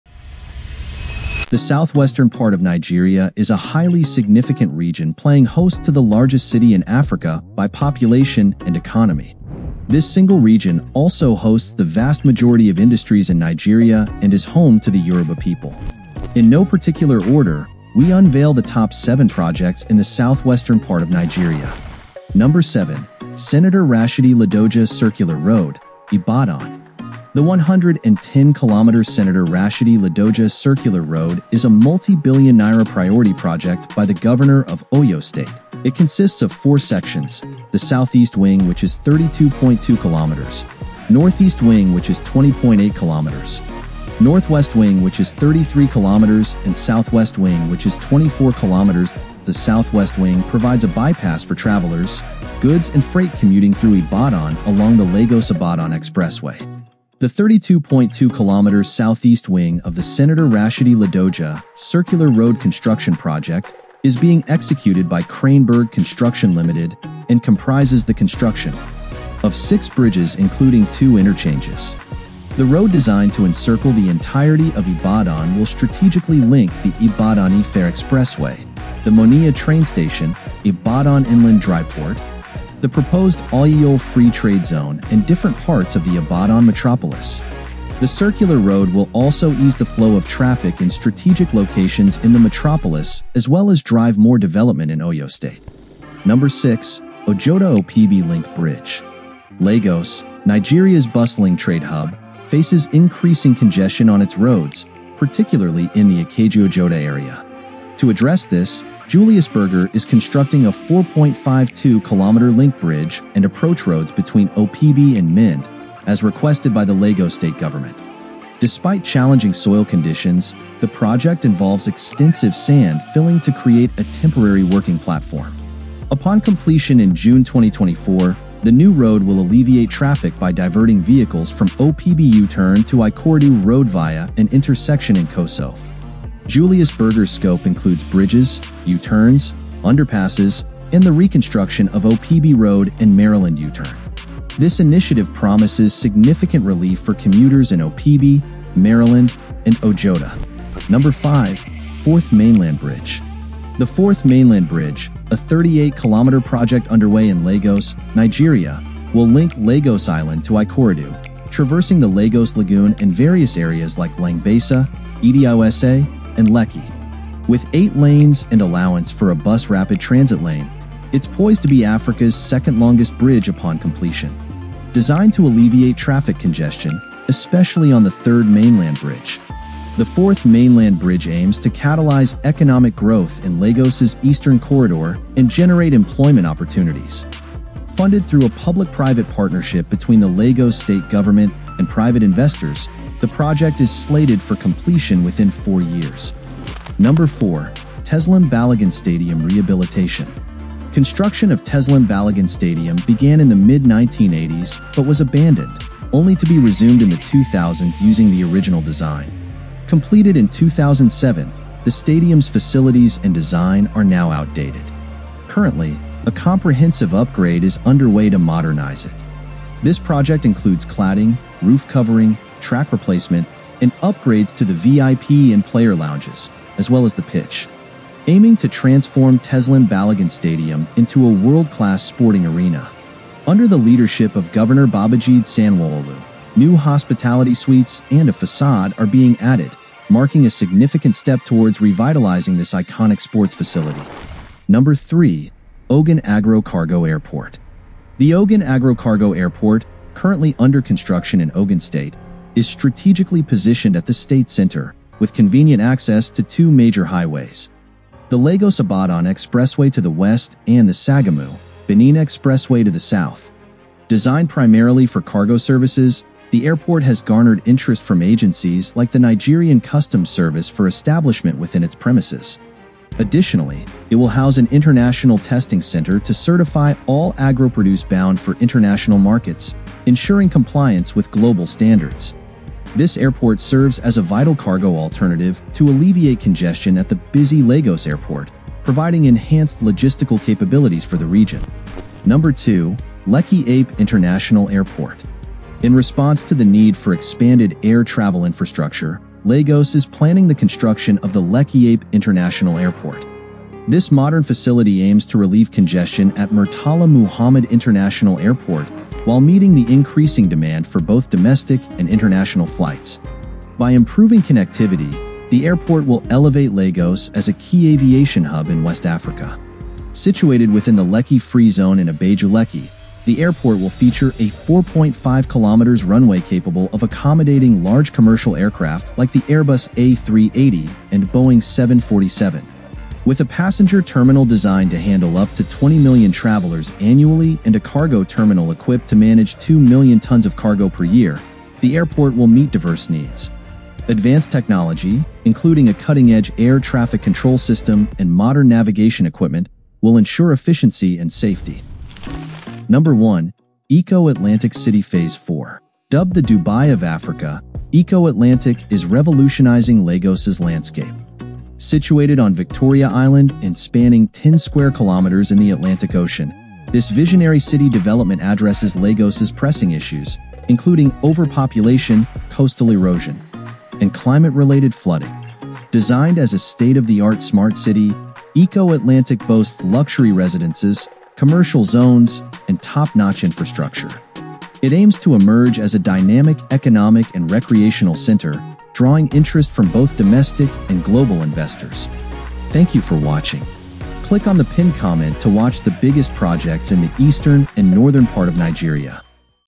Top 7 Must-See Projects in Southwestern Nigeria | Yoruba Land on the Rise!
The south western part of Nigeria is a highly significant region playing host to the largest city in Africa by population and economy. The region also host the Ibadan which is the largest city by land mass in Nigeria.

This single region also hosts the vast majority of industries in Nigeria and is home to the Yoruba people.

a lot of mega projects are scattered across the region particularly in Lagos, Ibadan and Ogun state.

in no particular order, we unveil the top 7 projects in the south western part of Nigeria.

TIMESTAMPS
00:00 Intro
00:21 Ibadan Circular Road
1:35 Ojota Opebi Link bridge
2:30 4th mainland bridge
3:16 Teslim Balogun stadium Rehabilitation
4:03 Ogun Agro Cargo Airport
4:51 Lekki Epe International Airport
5:51 Eko atlantic City Phase 4

Video Credit
Theniyifagbemi
Stephen Ndukwu
Eko atlantic city